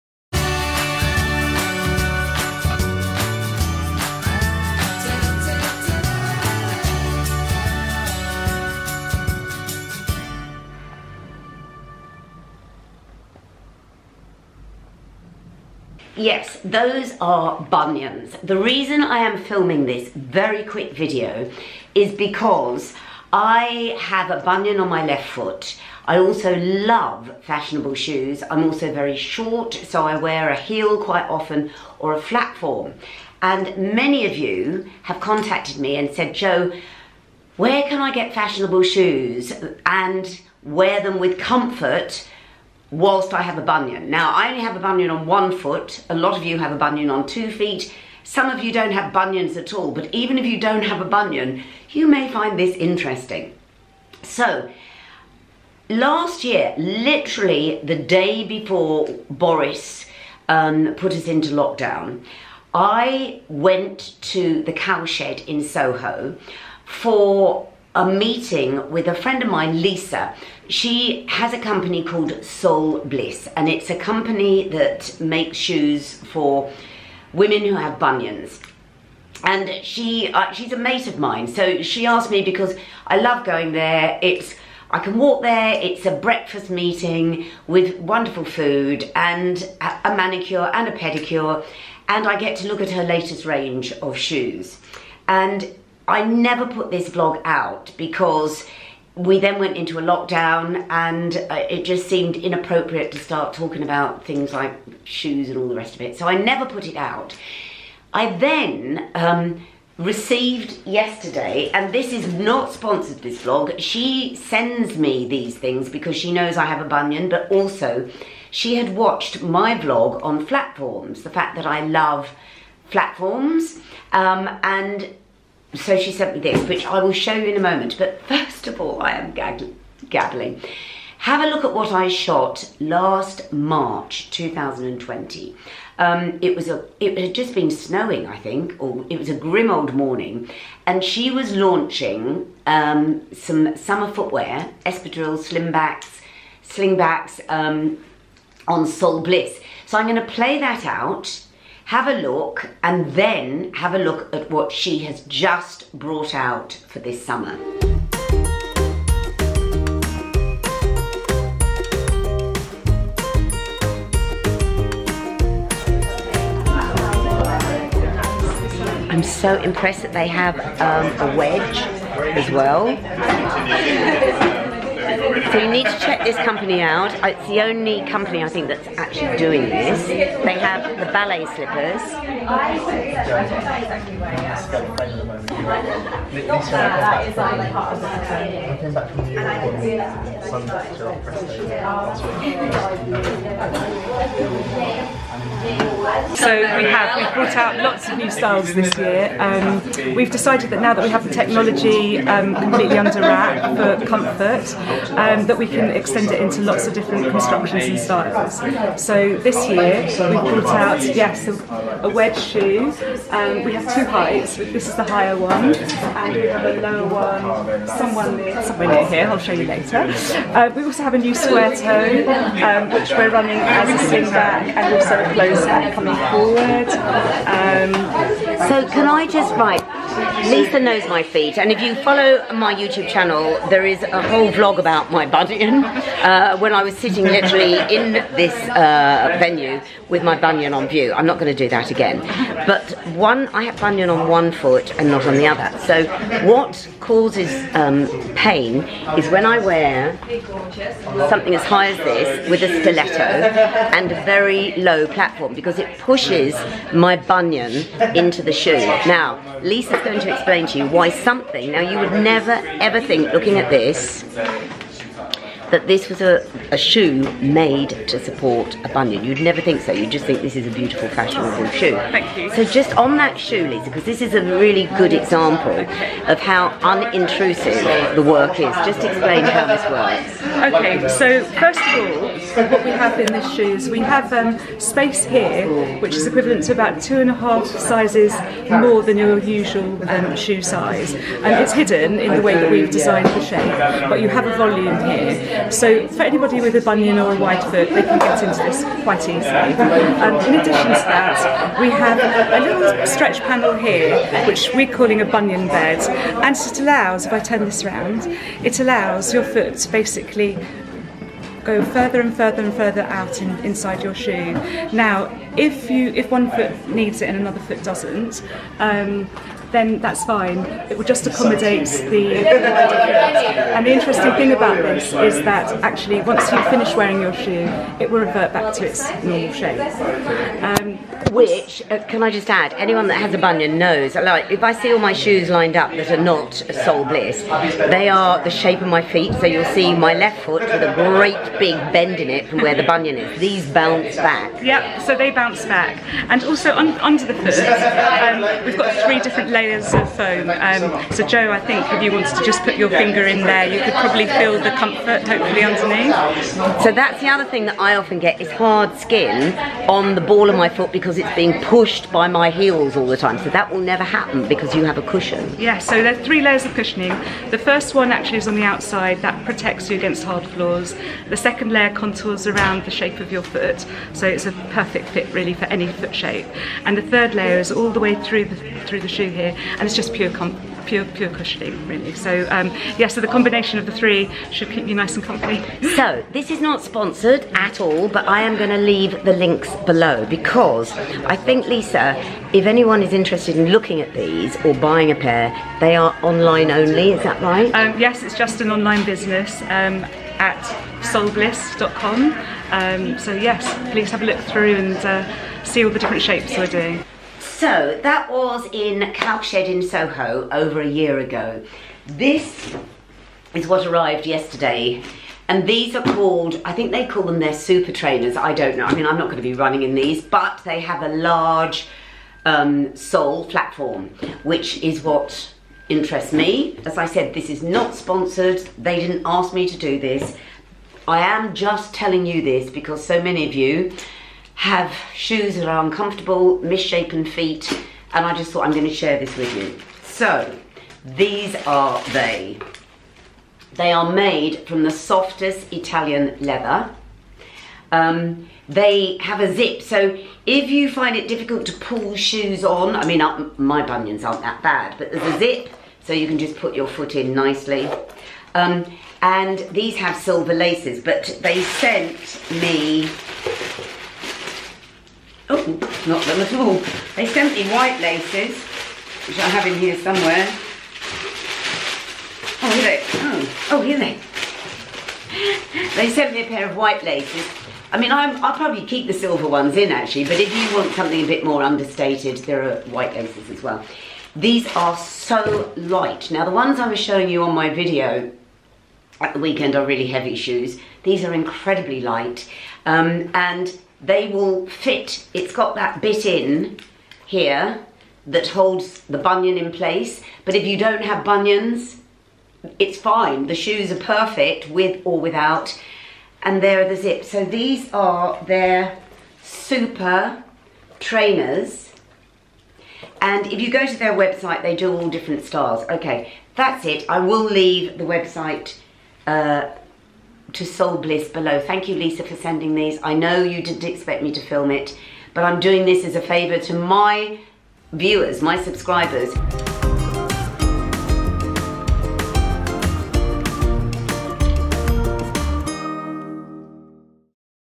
SUMMER SHOES FOR PROBLEM FEET | MID WEEK MINX
I have a bunion on my left foot. It came with middle age. Many of you admit to the same and ask how to enjoy fashionable footwear without experiencing pain. Well, Lisa is the founder of a groundbreaking company. She has a list of impressive clientele. Check the shoes out… and enjoy!

(Can also be found on iTunes!)